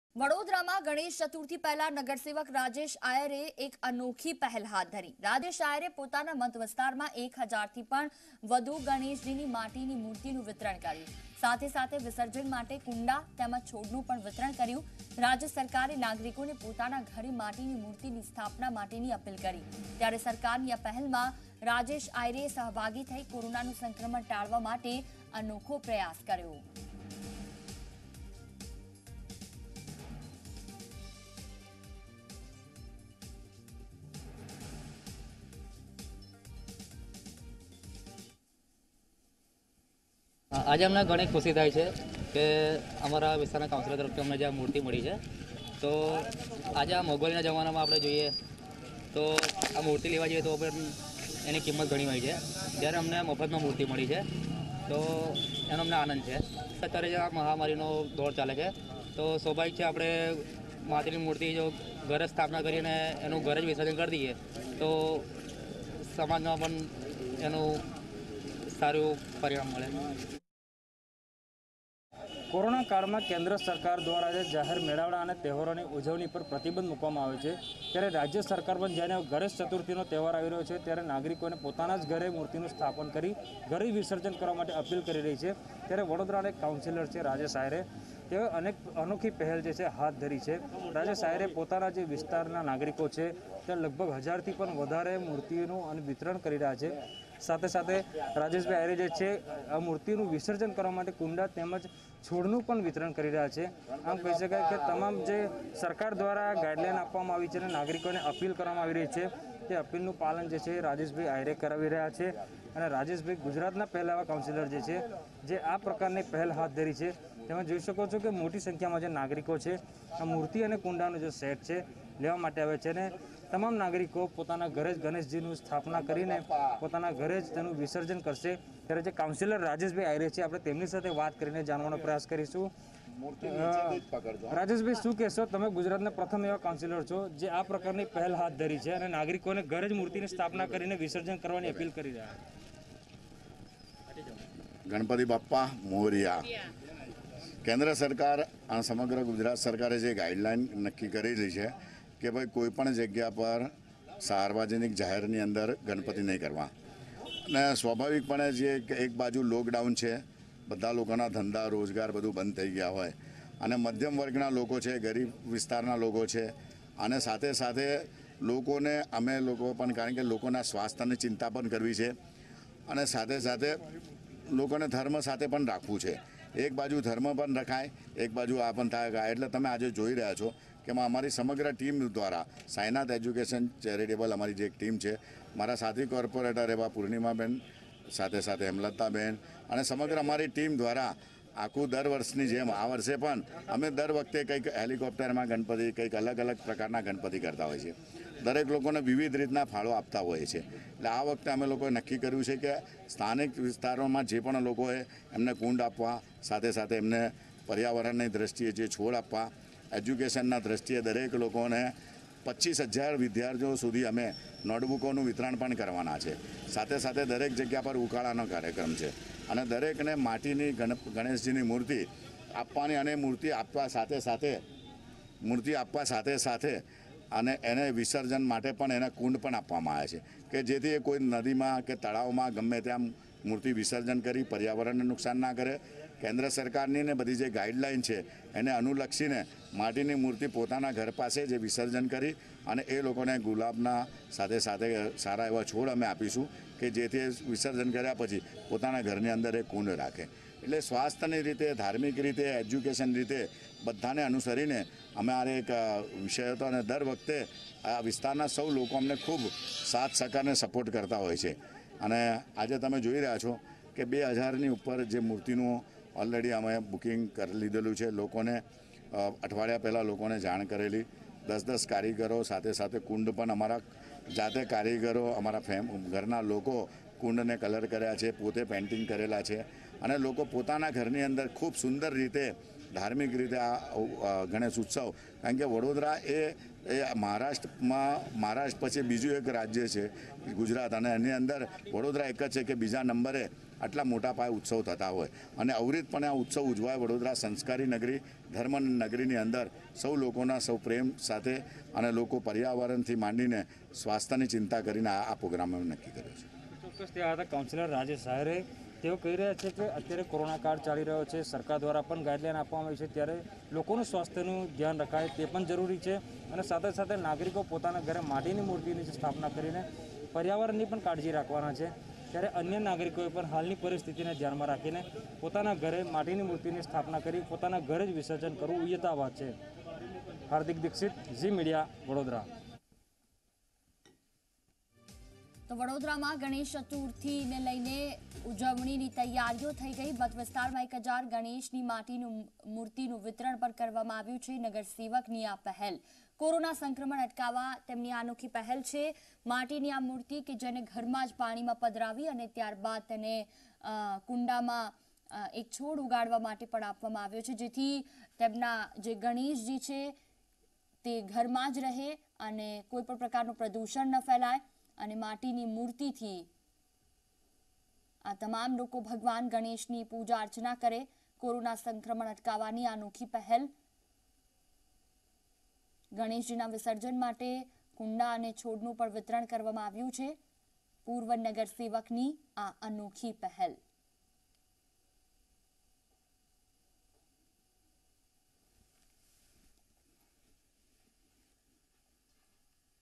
Vadodara માં Corona virus ના સંક્રમણ અટકાવવા નગરસેવકની અનોખી પહેલ | Ganesh Chaturthi | Gujarati news
વડોદરામાં કોરોના સંક્રમણ અટકાવવા નગરસેવક દ્વારા અનોખી પહેલ કરવામાં આવી છે. ગણેશજીના વિસર્જન માટે કુંડાં તેમજ છોડનું પર વિતરણ કરવામાં આવ્યું છે. મત વિસ્તારમાં 1 હજાર ગણેશજીની માટીની મૂર્તિનું વિતરણ કરવામાં આવ્યું છે.